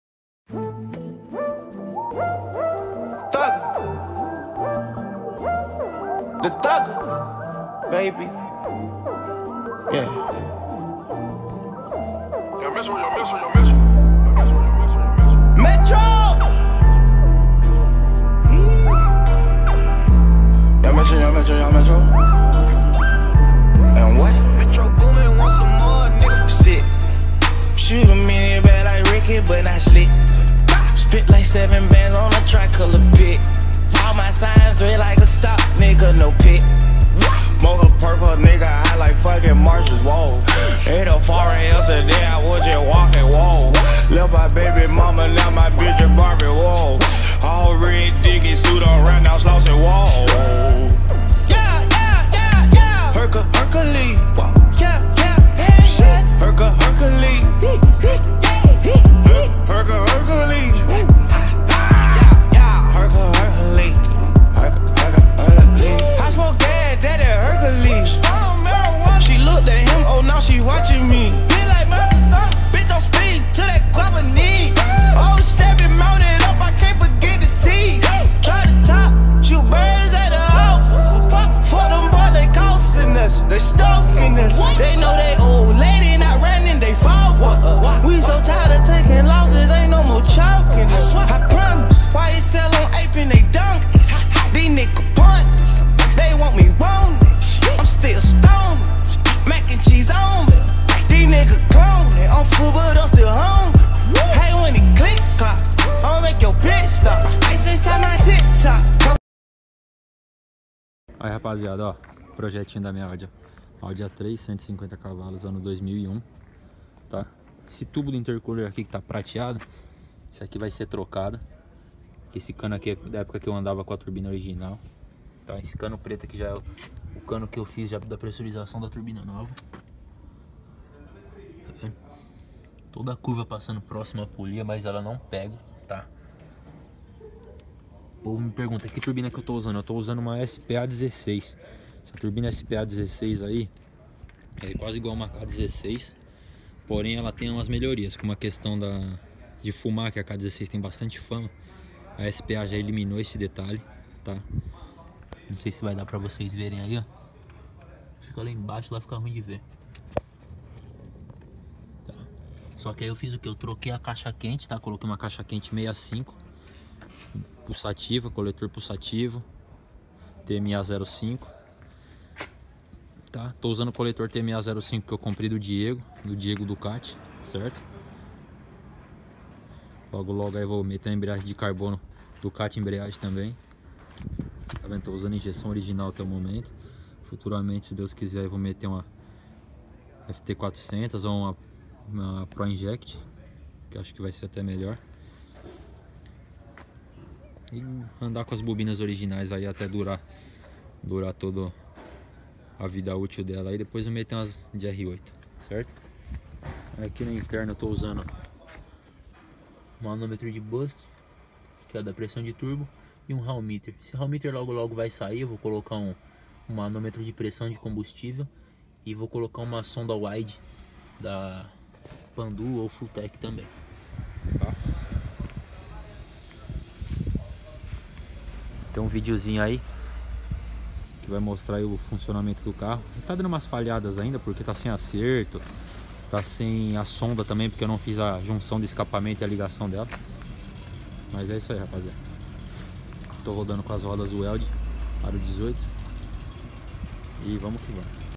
AUDI A3 k16 “Rodas weld”
Meu projeto de preparação
Audi a3 150cv
Itens modificados:

Bomba dinamica 12bar
Intake de 2,5”
Filtro
Maf 3”
Turbina SPA16 eixo 49 rotor 46mm
Caixa quente .65 pulsativa
Coletor tma 05
Bicos Bosch
Downpipe 3” inox
Escape 3” inox
Intercooler SPA pra 400cv
Blow off Tial
Válvula alivio Beep turbo
Regulador pressão 1:1
Combustível ALCOOL

Suspensao
Coil over jom BLUE
Buchas de bandejas oem AUDI

Falta
REMAP OVERSPEED
BOBINAS AUDI R8
WIDEBAND FUELTECH